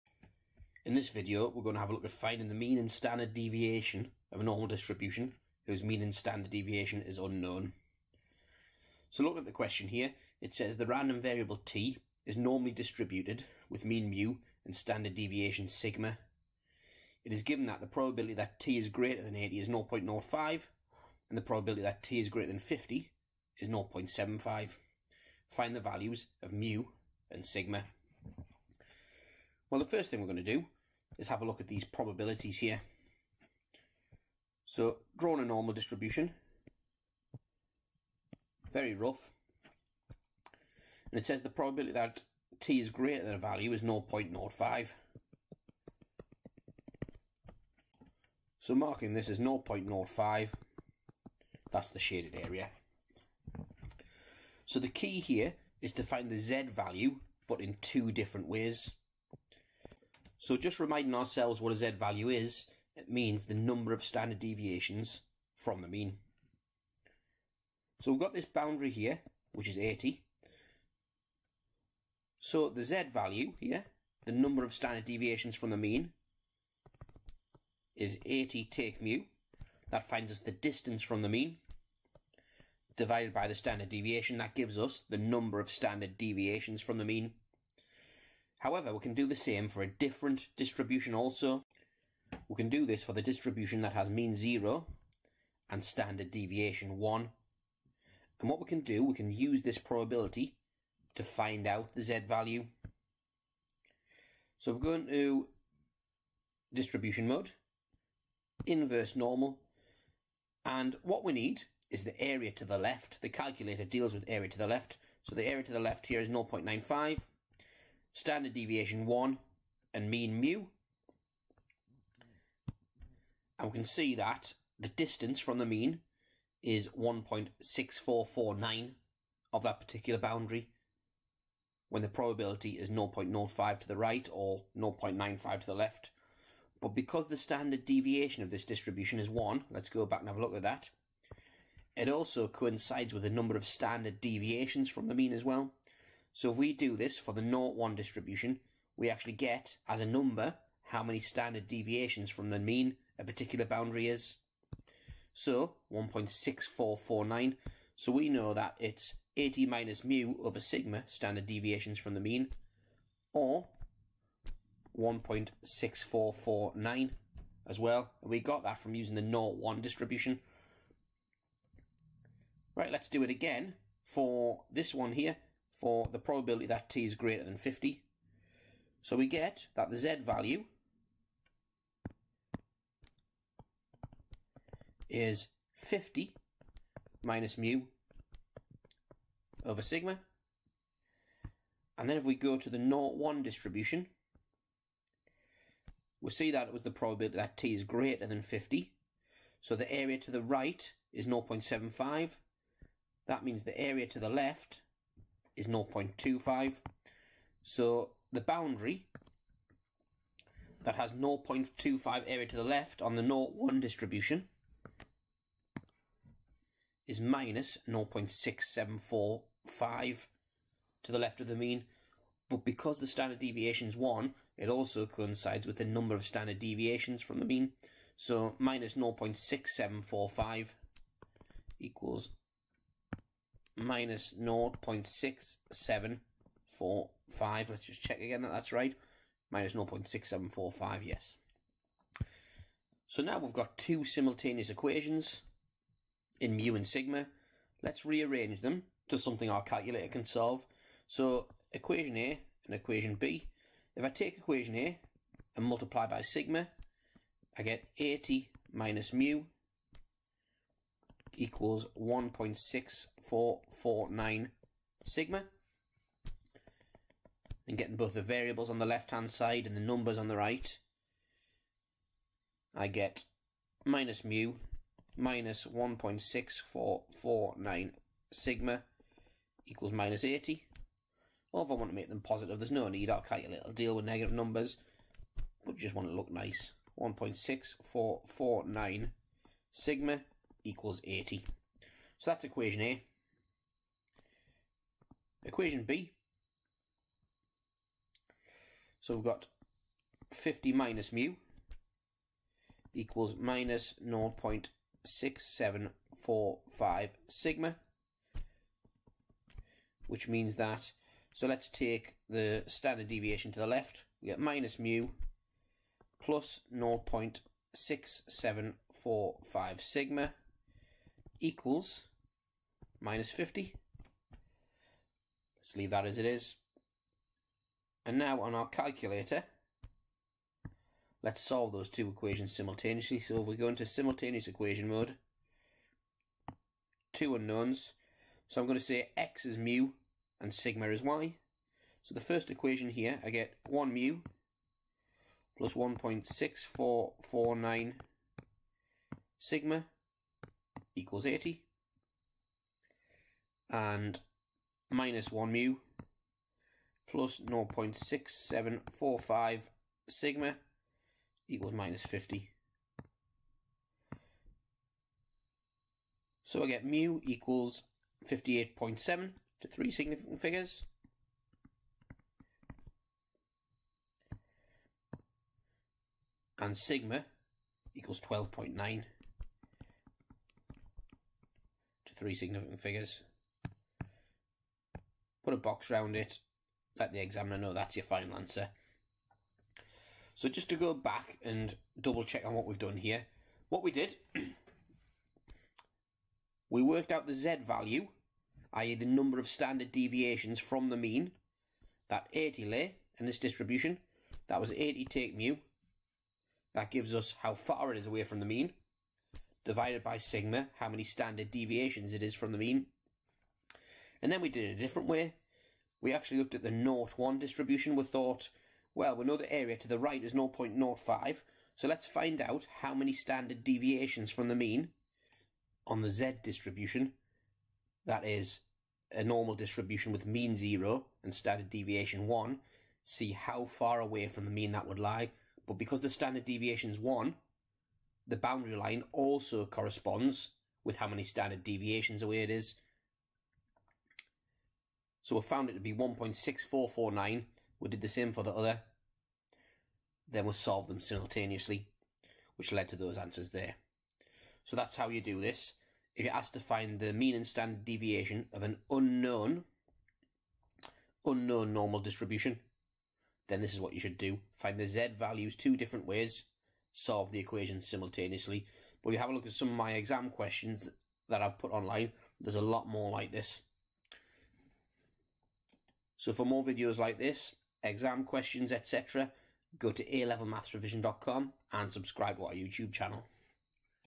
Normal Distribution - Finding Mean and Standard Deviation Normal Distribution [Year 2 Maths (Stats)]
An A Level Maths Revision tutorial on how to find the mean and standard deviation of a normal distribution when the mean and standard deviation is unknown. For more information on my one-to-one tutoring please